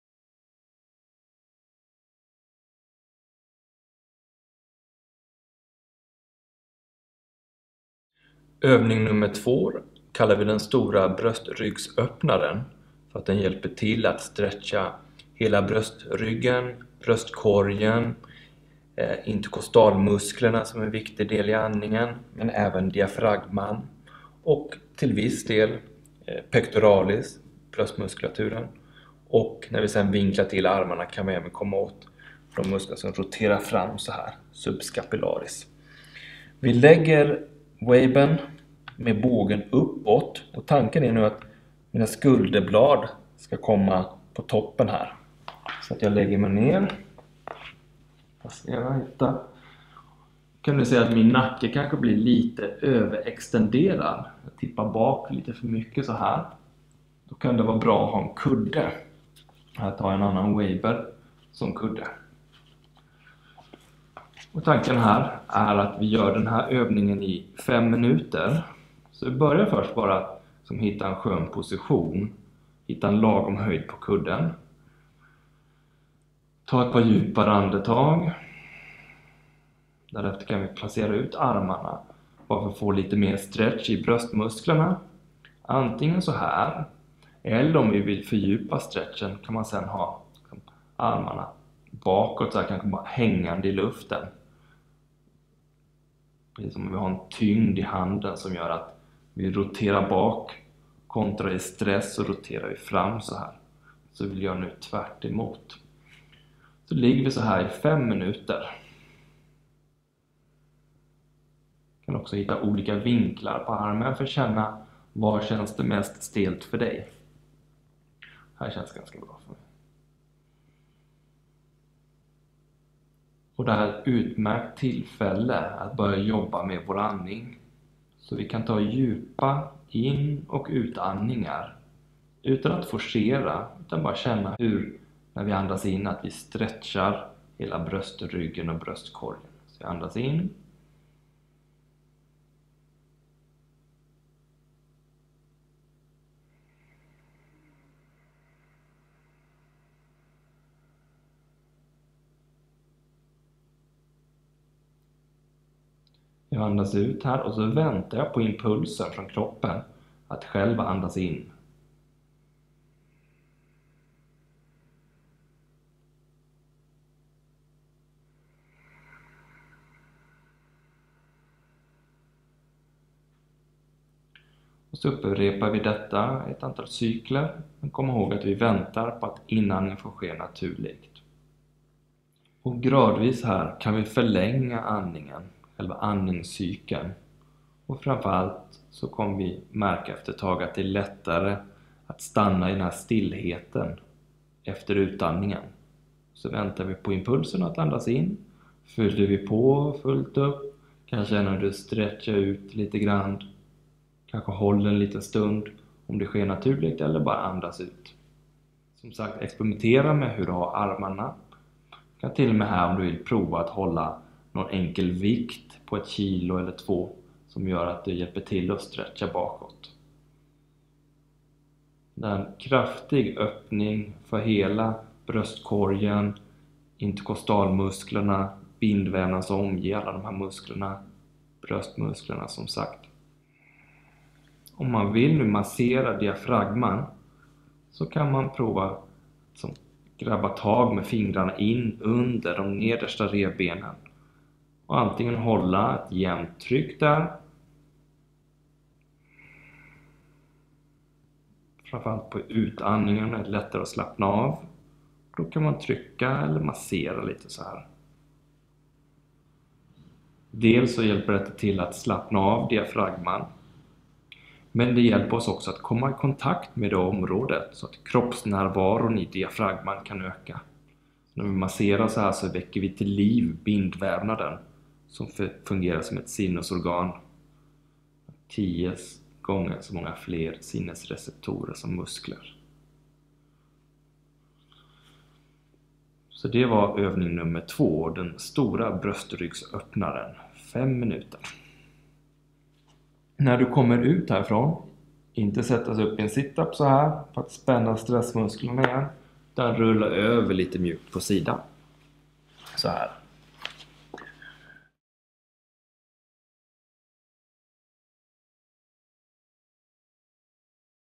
Wabe Office 10 Dagars Utmaning - Den Stora Bröstryggsöpppnaren (övning 2)
I Wabe Office använder vi pressurredskapet Waber för att släppa på spänningar i bröstryggen och nacken. Det är vanligt att musklerna i de här områdena spänns när vi till exempel sitter framför en dator och/eller under en längre tid upplever stress. Spänningar i dessa regioner kan till exempel orsaka spänningshuvudvärk, försämrad andningsförmåga och dålig hållning.

I vår 10 dagars utmaning gör du ett 12 minuters träningsprogram 2 gånger per dag under 10 dagar (totalt 240 min). Programmet består av 4 olika övningar för bröstrygg och nacke. Den Stora Bröstryggsöpppnaren är den andra övningen som görs under 5 minuter.